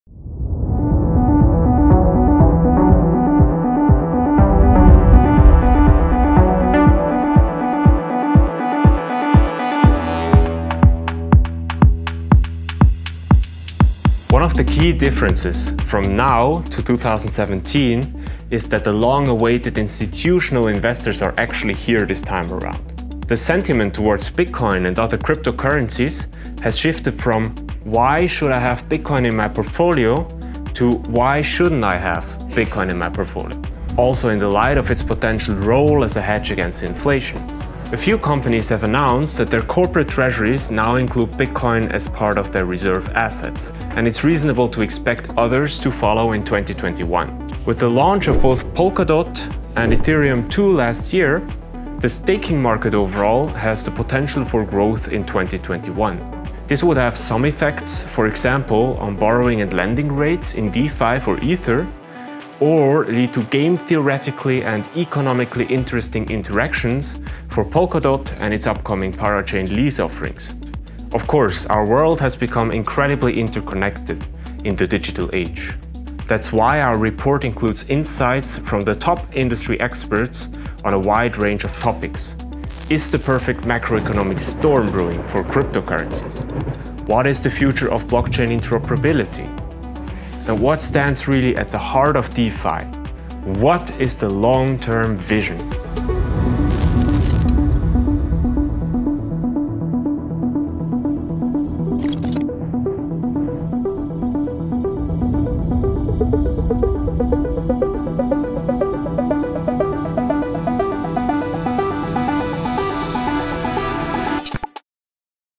Bitcoin Suisse Crypto Outlook 2021
An in-depth look at the most important trends and topics in the world of crypto and blockchain in 2021 from Dr. Raffael Huber of Bitcoin Suisse Research, with contributions from Fatemeh Shirazi from the Web3 Foundation, Giles Keating, former Chief Global Economist at Credit Suisse Investment Banking, Roger Studer, Bitcoin Suisse Series A lead investor - and more.